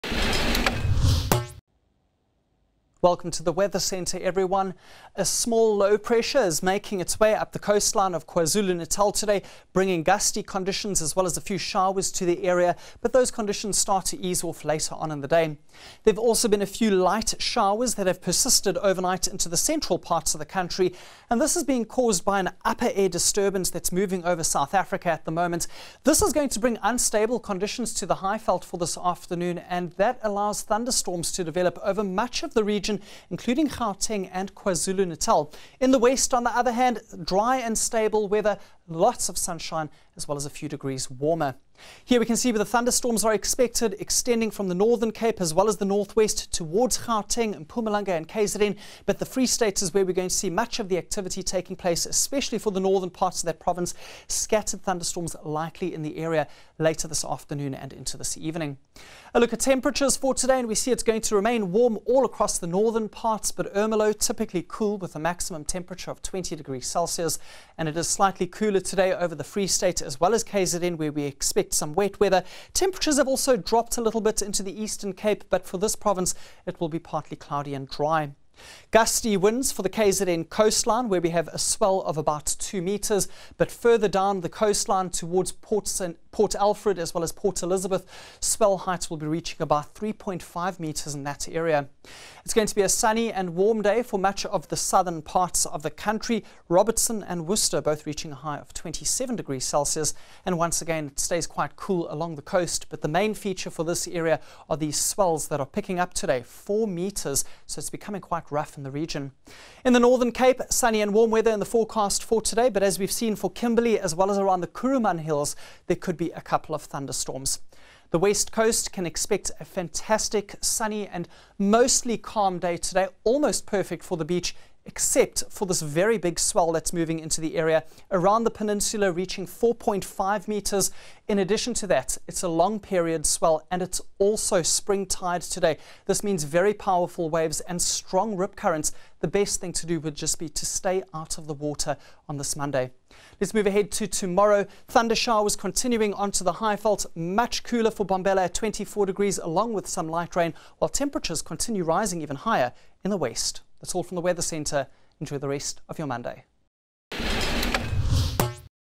eNCA Weather Morning - 30 April 2018
eNCA's weather anchor Luis Fernandes with the latest weather updates.